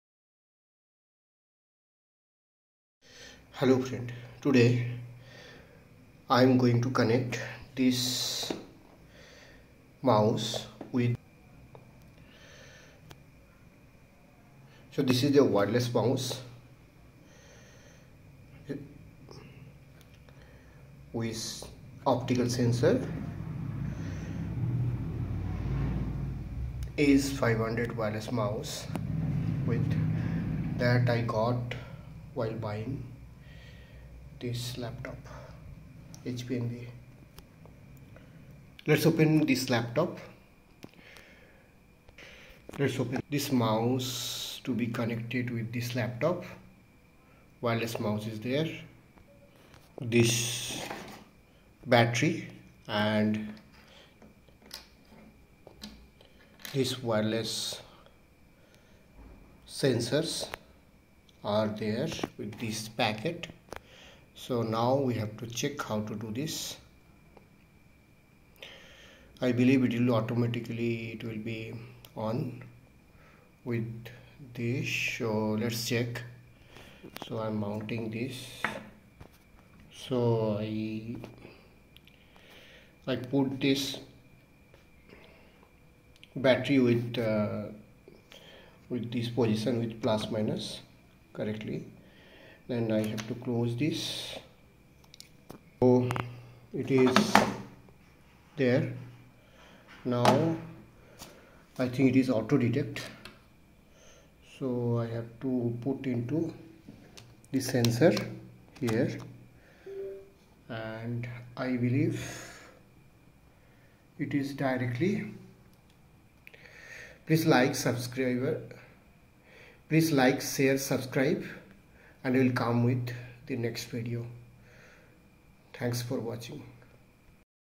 How to Connect a Wireless Mouse | Step-by-Step Guide
Learn how to easily connect a wireless mouse to your computer with our comprehensive step-by-step guide. Whether you're a beginner or an experienced user, this tutorial will walk you through the process of pairing and setting up your wireless mouse. Say goodbye to cables and enjoy the convenience of a wireless mouse. Watch now and get started!